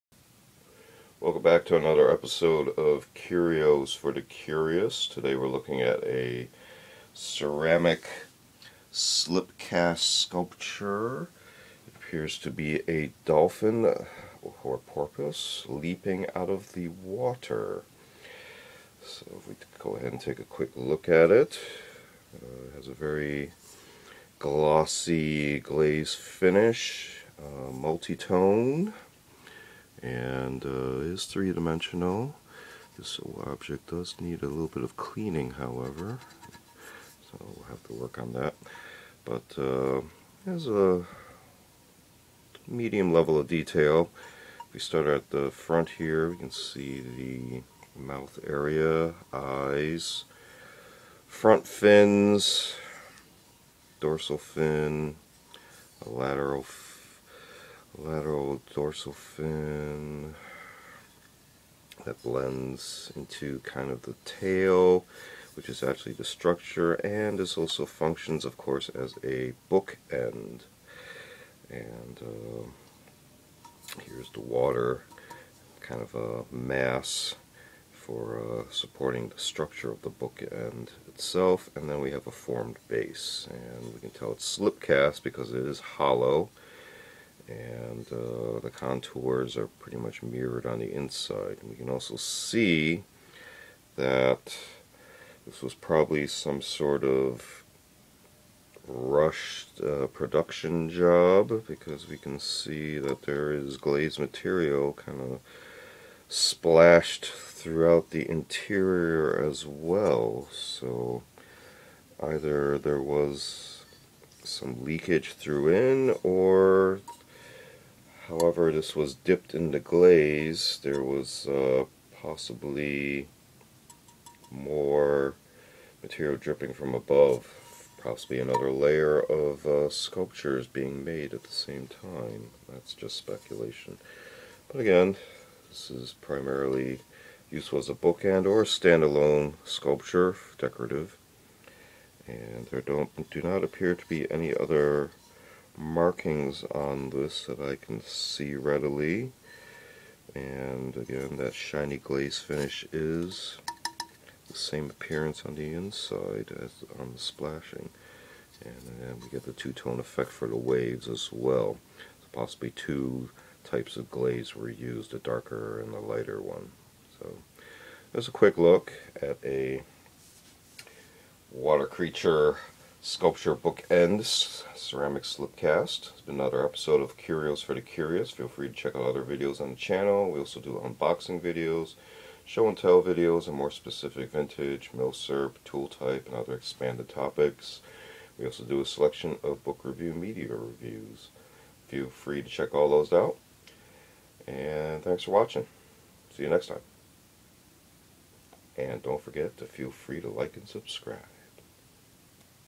CURIOS for the CURIOUS 34: Ceramic Slip-Cast Leaping 🐬Dolphin🐬 Bookend (Phocoenidae / Odontoceti).
CURIOS for the CURIOUS [34]: Ceramic Slip-Cast Leaping 🐬Dolphin🐬 Bookend (Phocoenidae / Odontoceti). Curiousities collected from around the planet known as SOL 3.